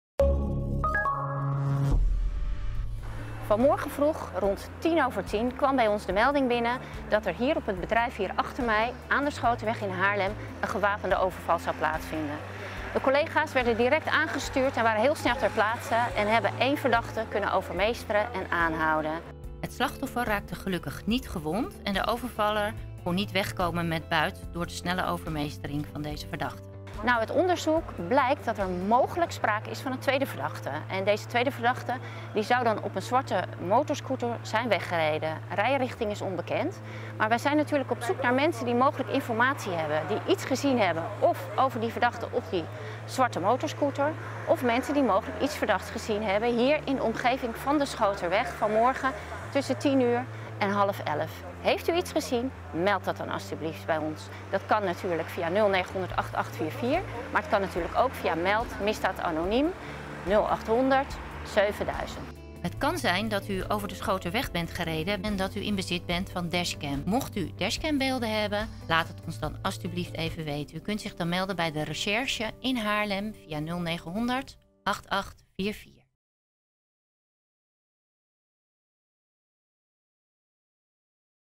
POLNH: Verdachte aangehouden bij gewapende overval Haarlem
Maandagochtend heeft de politie direct na een snelle overvalmelding een verdachte kunnen aanhouden.